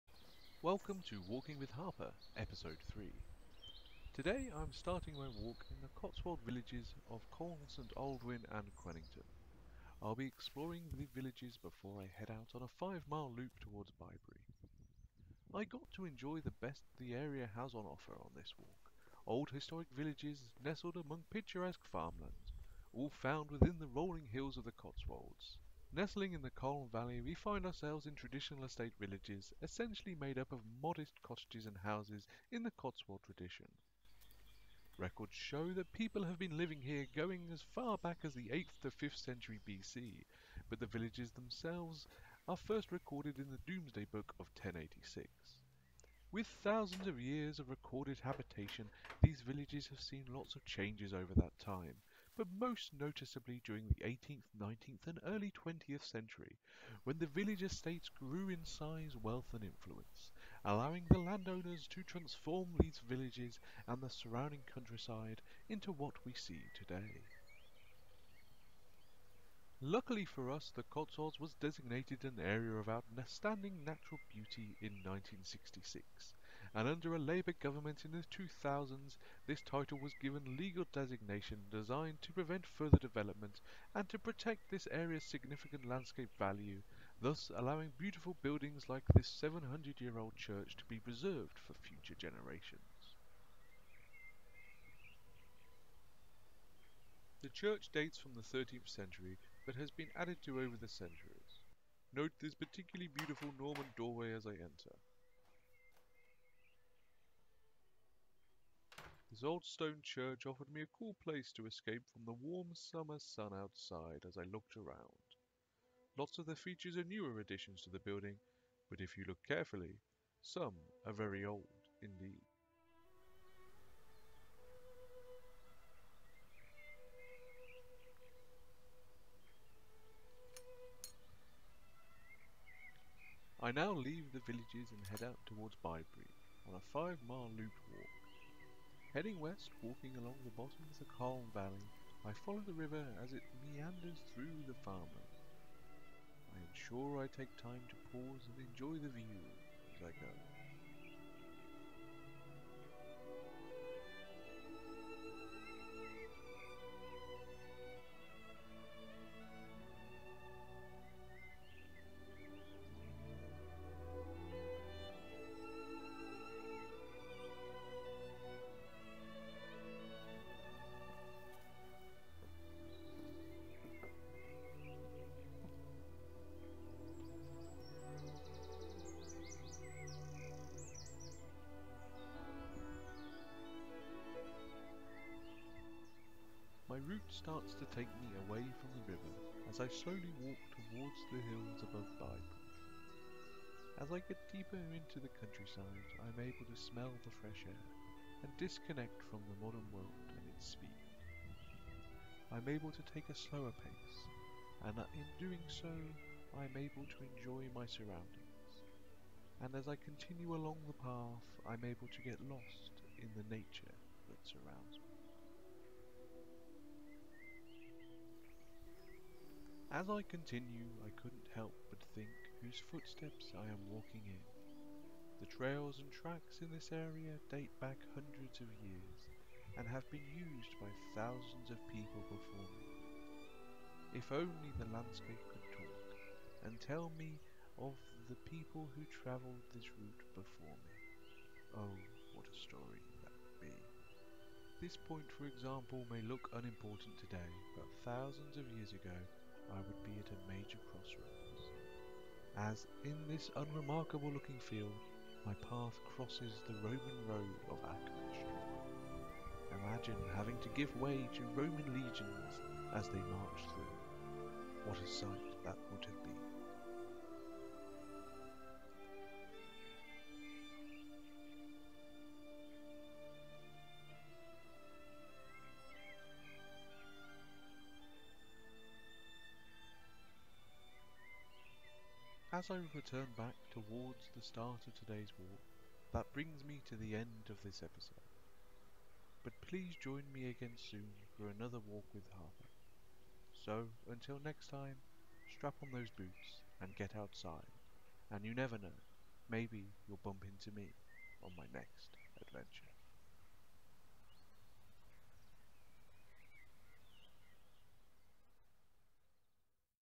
Walking with Harper - Ep 3. Coln St. Aldwyns & Quenington
In episode 3 I take you on a walk in the Cotswold villages of Coln St. Aldwyns and Quenington then explore the surrounding countryside.